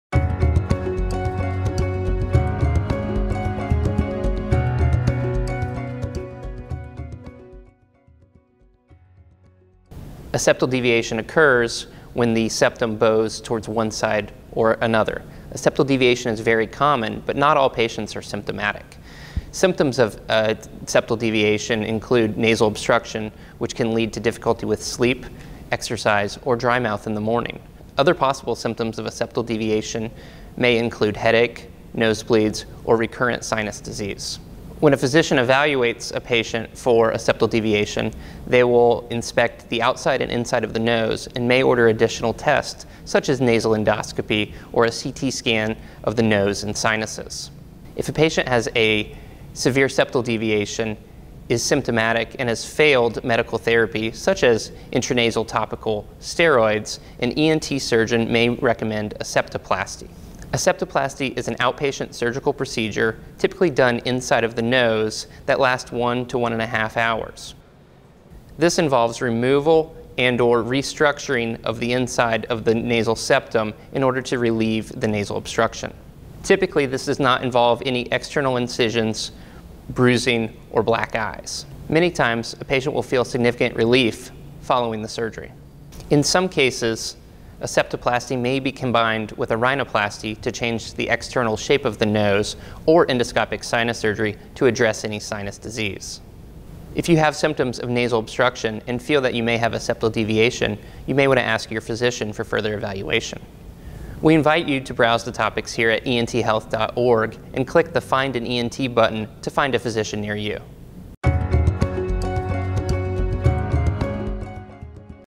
Deviated Septum
Dr. Adam P. Campbell discusses septal deviations, and how otolaryngologist-head and neck surgeons are uniquely trained to provide treatment and advice on this subject.